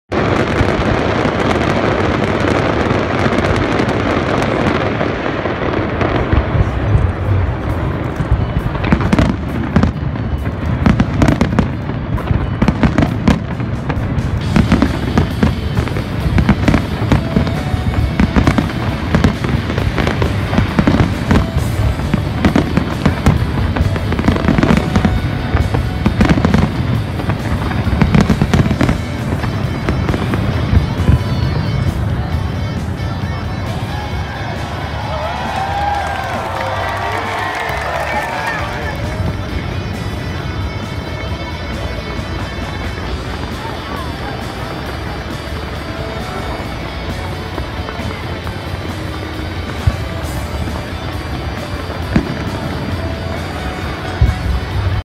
Brisbane Riverfire
iPhone 6s Plus footage of Brisbane riverfire 2015.